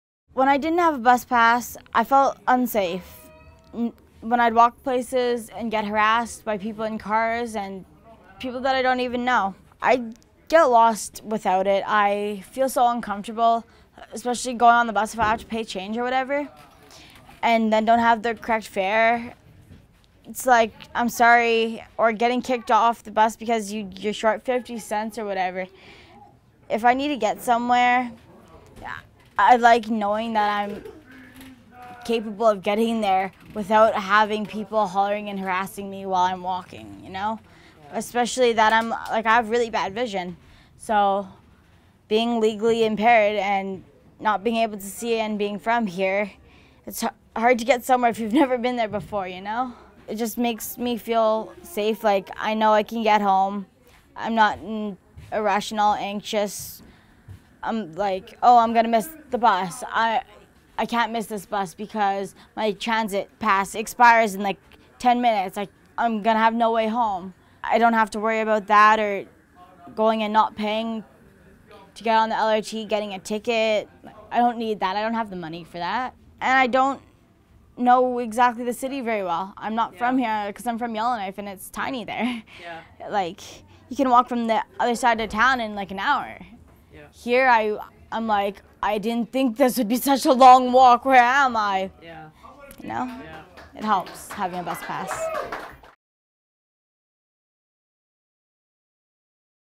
Transit fines
Saidee Gosselin talks about the importance of being able to afford a bus pass and how she uses it for her own safety.